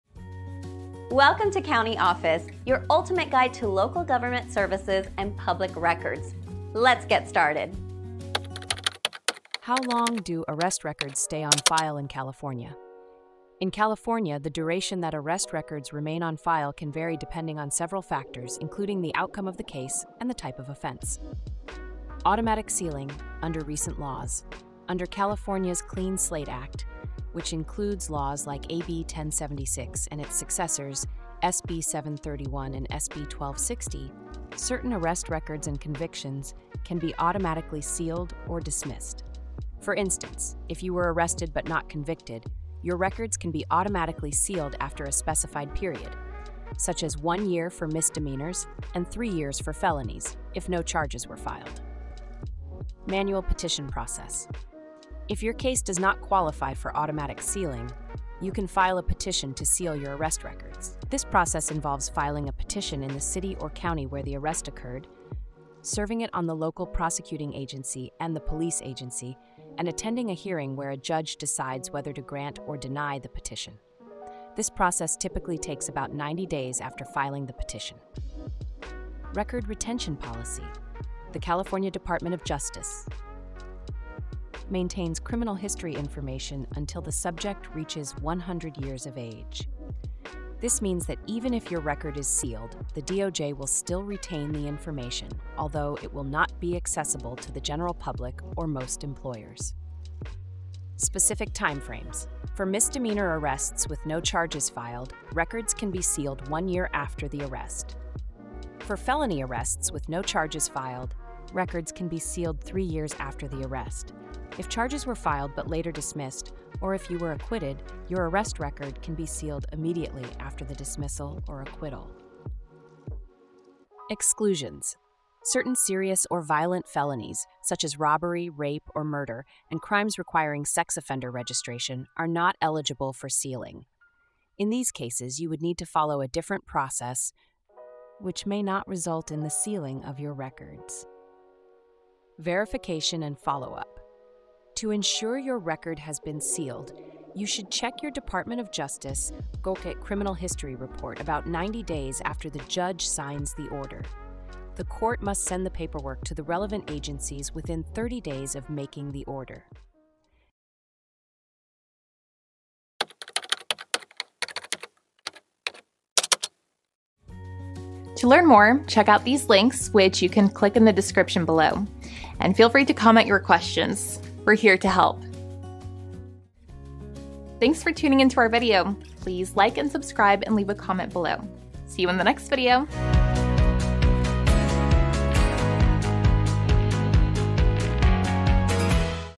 How Long Do Arrest Records Stay on File in California? | CountyOffice.org
How Long Do Arrest Records Stay on File in California? Discover everything you need to know about how long arrest records stay on file in California in our latest video. Learn about the Clean Slate Act and the manual petition process for sealing arrest records. Find out about the record retention policy of the California Department of Justice and specific time frames for sealing records based on different scenarios. Uncover exclusions for certain serious felonies and the verification process to ensure your record is sealed. Stay informed and protect your rights by watching this informative video now!